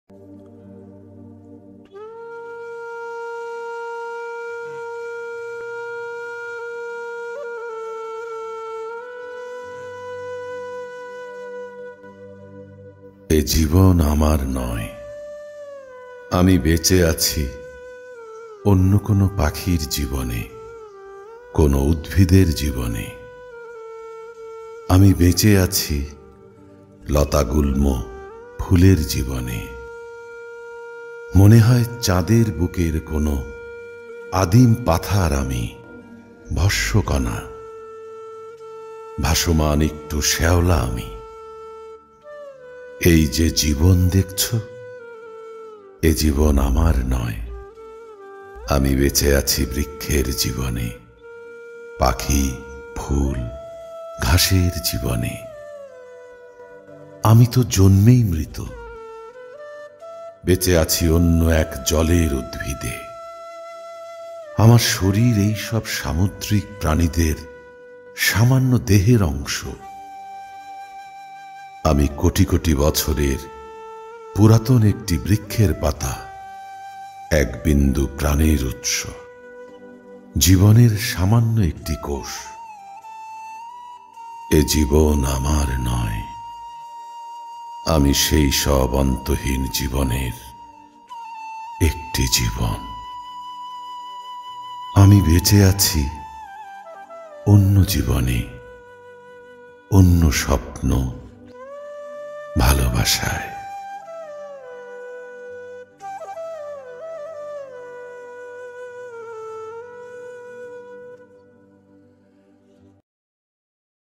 E JIBON AMAR NOY (এ জীবন আমার নয়) POET: MOHADEB SAHA (মহাদেব সাহা) কণ্ঠেঃ মোঃ আব্দুর রহিম
POEM: E JIBON AMAR NOY
             (এ জীবন আমার নয়)
POET:  MOHADEB SAHA
             (মহাদেব সাহা)
কণ্ঠেঃ    মোঃ আব্দুর রহিম
             (MD. ABDUR RAHIM)
আমি বেঁচে আছি অন্য কোনো পাখির জীবনে,
কোনো উদ্ভিদের জীবনে,
আমি বেঁচে আছি,  লতাগুল্ম-ফুলের জীবনে,
মনে হয়  চাঁদের বুকের কোনো
আদিম পাথার আমি, ভস্মকণা,
ভাসমান একটু শ্যাওলা আমি;
এই যে জীবন দেখছো  এ জীবন আমার নয়,
আমি বেঁচে আছি  বৃক্ষের জীবনে,
পাখি, ফুল, ঘাসের জীবনে ।
আমি তো জন্মেই মৃত,
বেঁচে আছি অন্য এক , জলের উদ্ভিদে -
আমার শরীর এইসব  সামদ্রিক প্রাণীদের
সামান্য দেহের অংশ,
আমি কোটি কোটি বছরের
পুরাতন একটি বৃক্ষের পাতা
একবিন্দু প্রাণের উৎস,
জীবনের সামান্য একটি কোষ;
আমি সেইসব অন্তহীন জীবনের একটি জীবন,
আমি বেঁচে আছি অন্য জীবনে,
অন্য স্বপ্ন- ভালোবাসায় ।